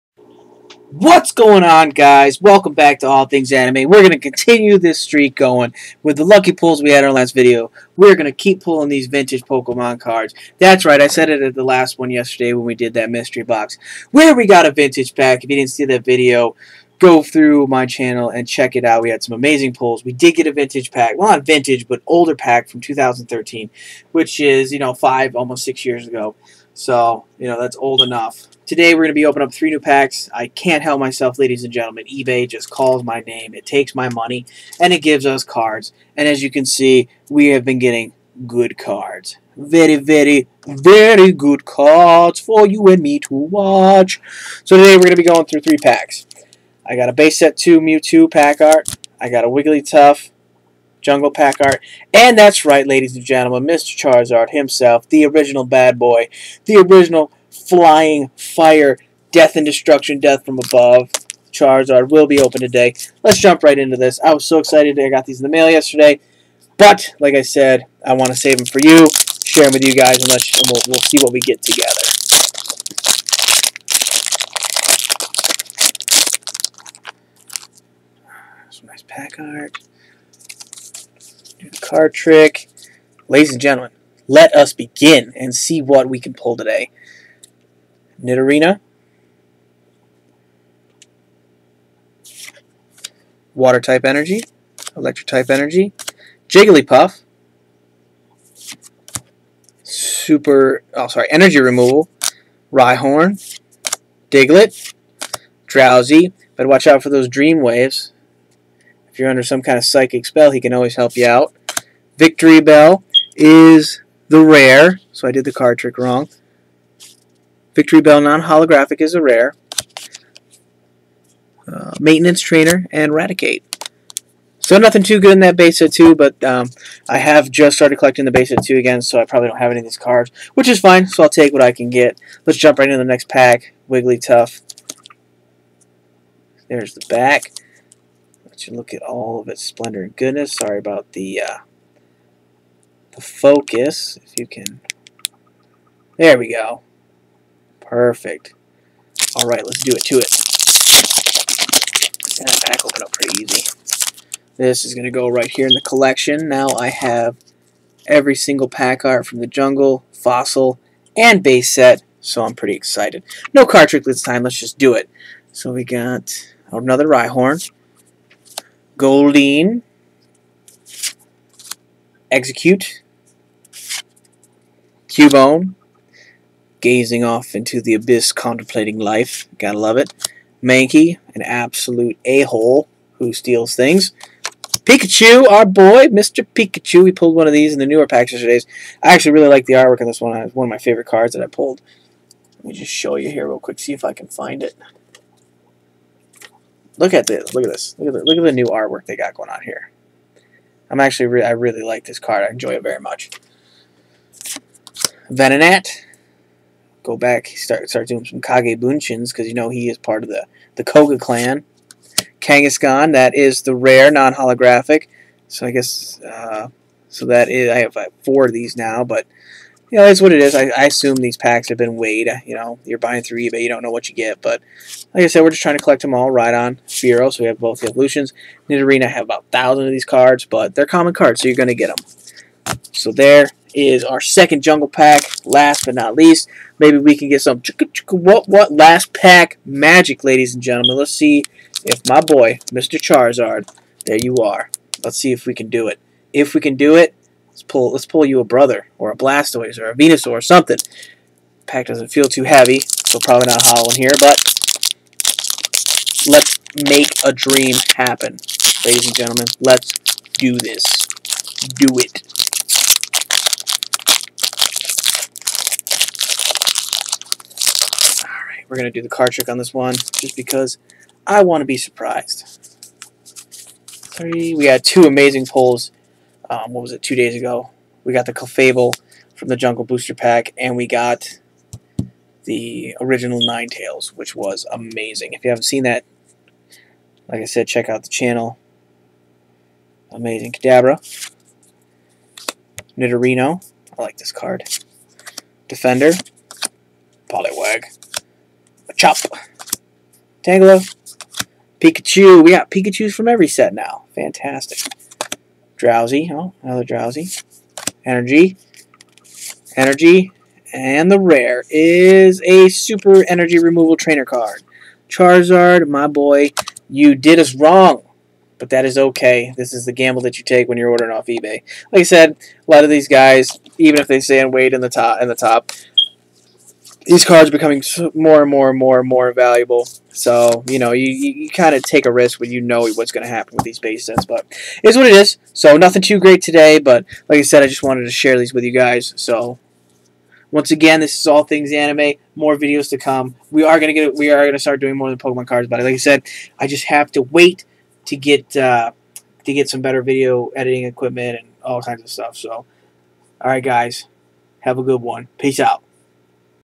Even more VINTAGE cards being opened!!!!!!!!!
Base set 2 jungle and base set pokemon card opening